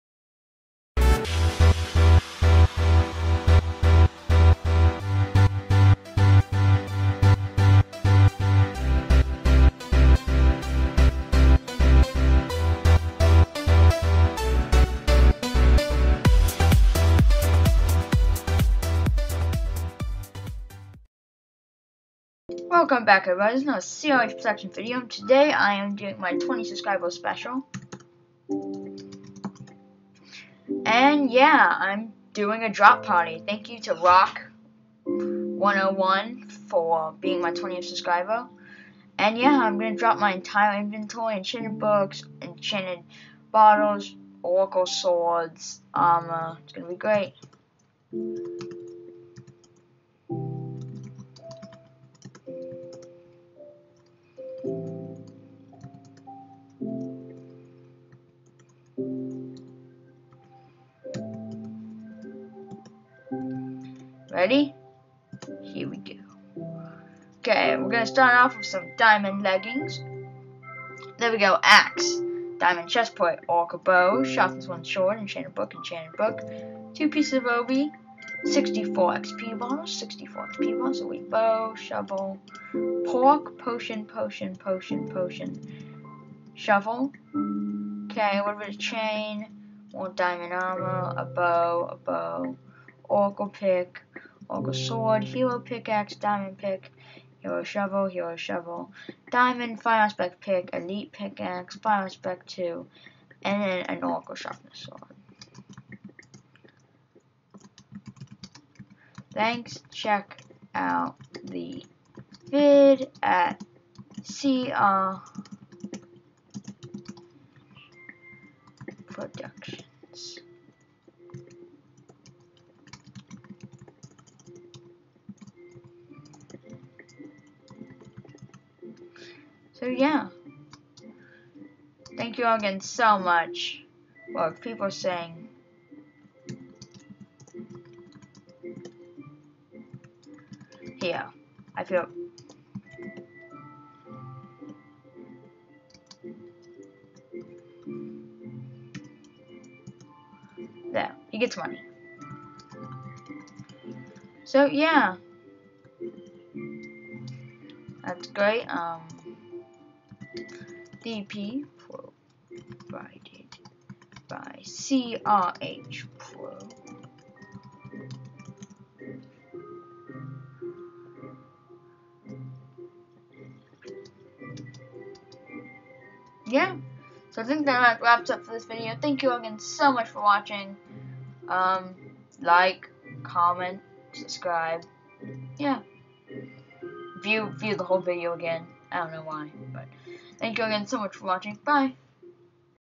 20 subscriber factions dp!
I hope you all enjoy my video or got something. I am playing on pvp.thearchon.net factions red.